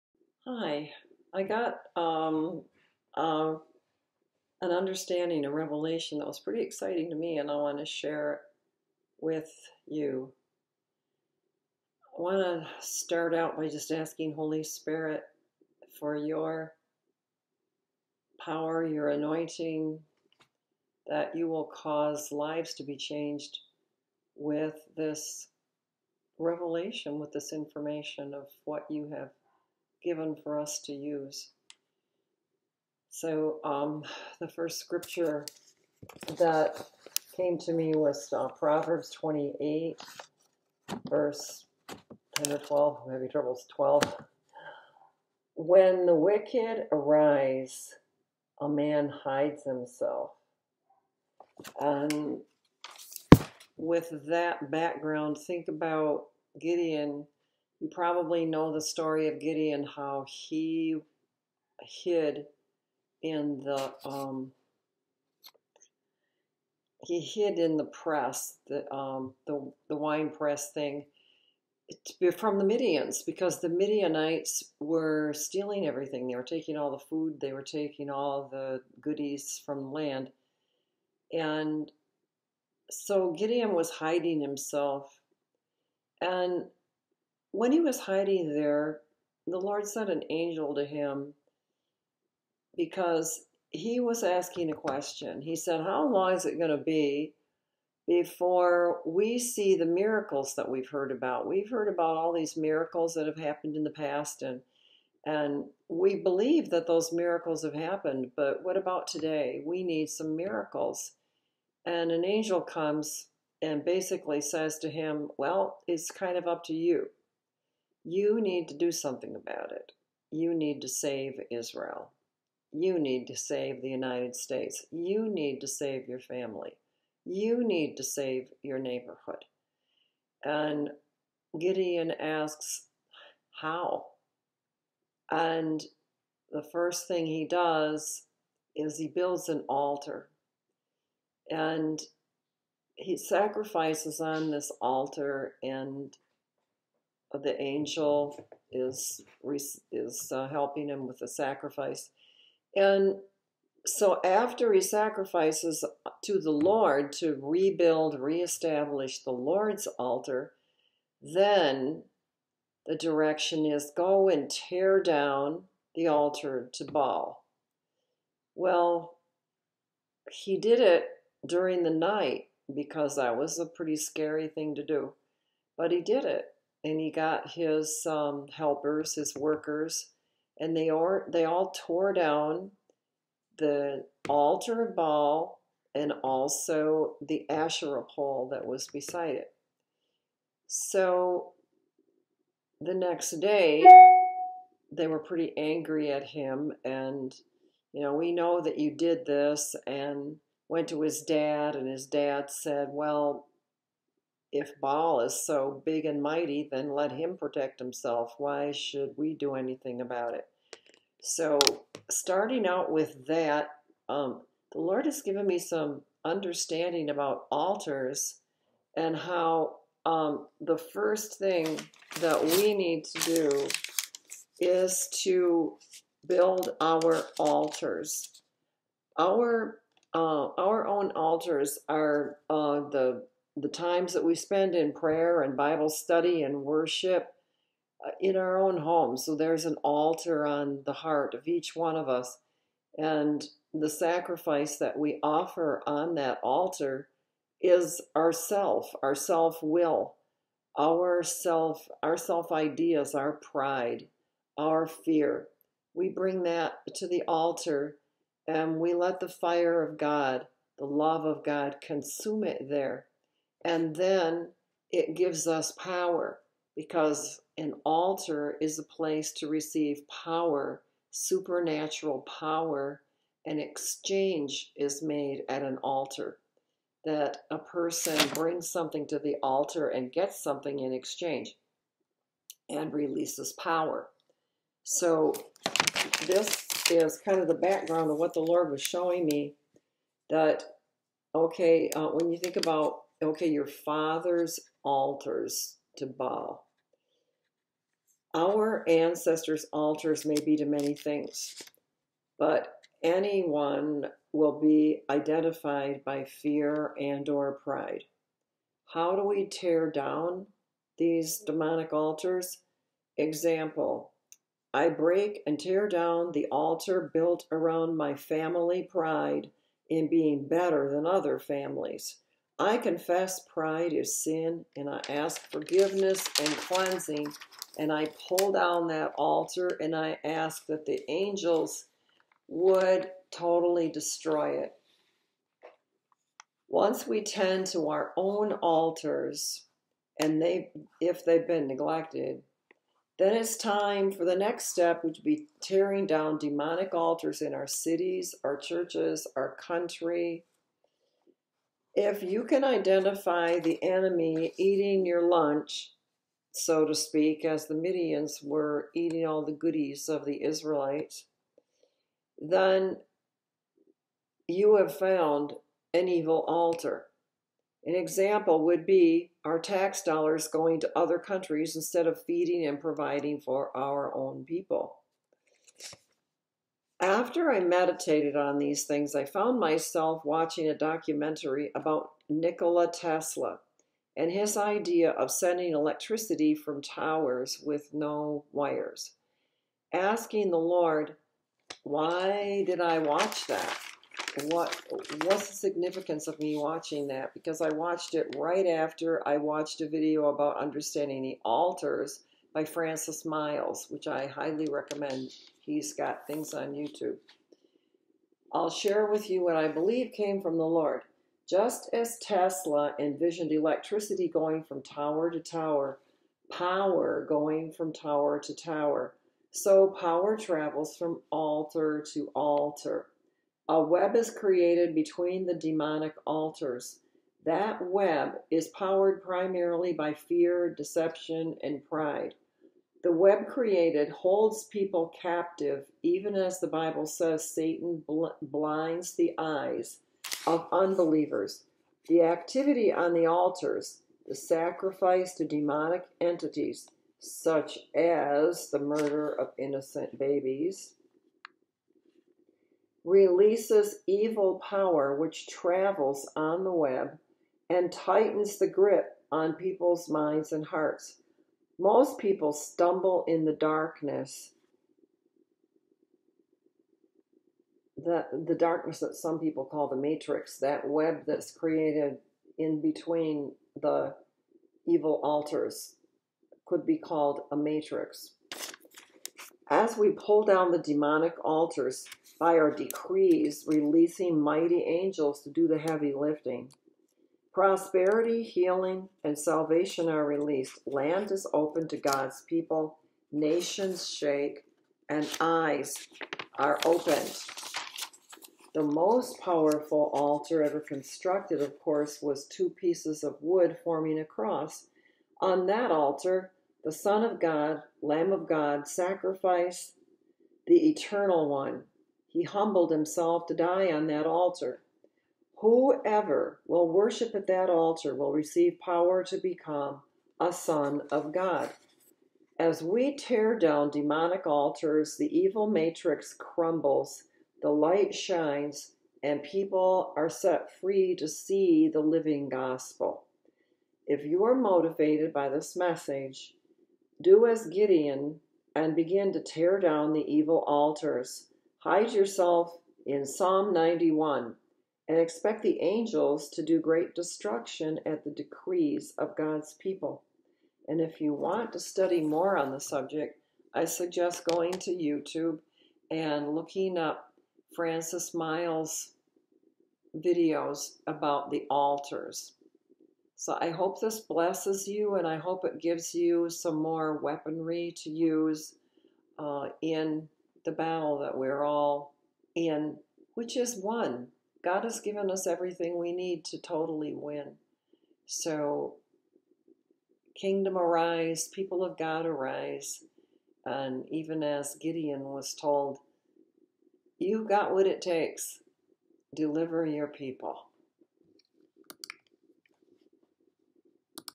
Altar Revelation for Change. Movie on 10 23 23 at 4 13 PM
Important information regarding the construction and maintenance of the matrix holding people captive